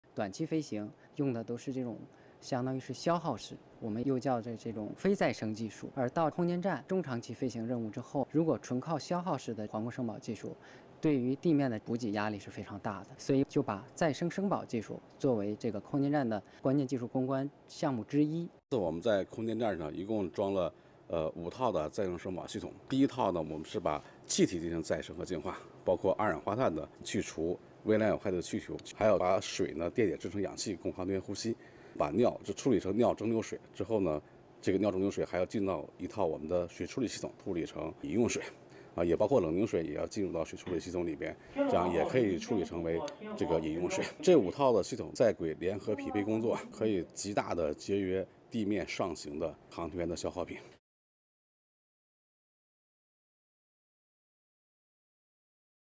China space station: How do regenerative life support systems work?
Astronauts depend on cutting-edge technology to regenerate vital resources such as air, water and other life support systems in outer space. The fundamental technology responsible for creating a livable environment is called environmental control and life support. So how do these systems work for China's space station?